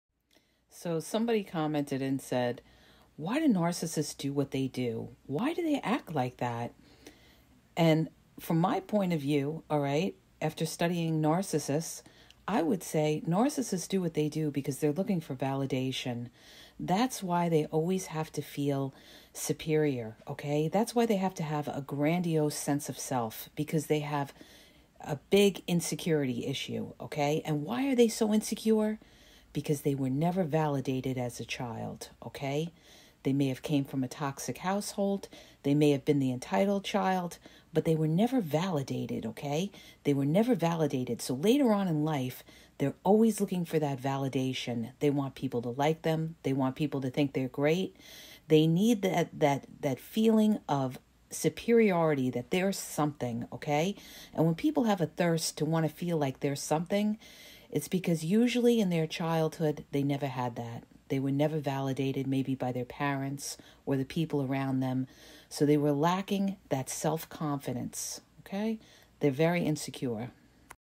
Why Do Narcissists Do What They Do?
This video talks about why narcissists do what they do.

To ask a question and get a video sent to you go

Read “He’s Not Into You-He’s Wasting Your Time” 👉🏼